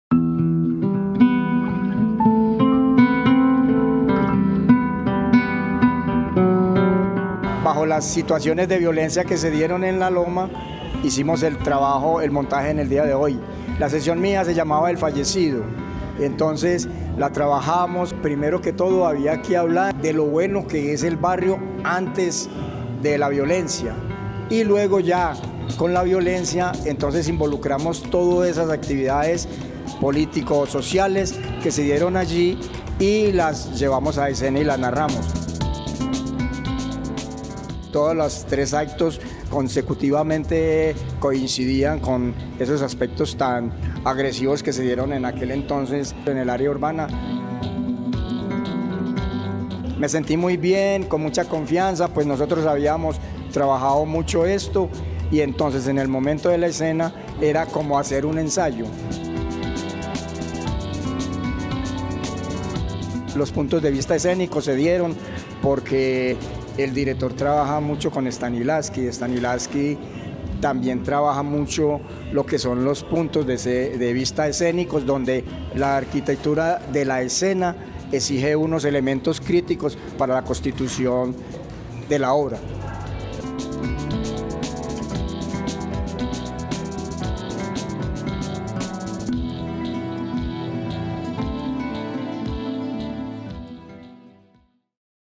¿Cómo el teatro nos ayuda a re-interpretar la realidad? 🤯
Junto a @AmbulantesDe y la Red de Creación Escénica, en el 2022 creamos rituales discursivos para expresar las historias de vida que necesitaban ser contadas, re-construidas y re-pensadas desde la poética de las artes escénicas 🎭

 ¡Gracias por este gran talento!